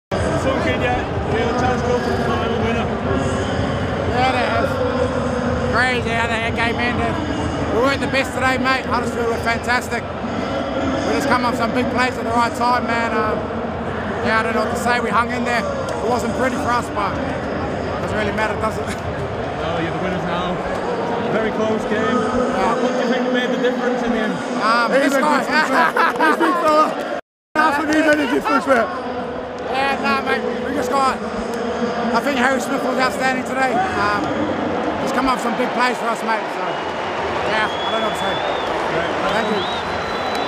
Thomas Leuluai Reaction | Betfred Challenge Cup Final
The immediate reaction ofThomas Leuluai (and John Bateman!) to winning the 2022 Betfred Challenge Cup...

Keep up with the latest Challenge Cup news and content across the following channels: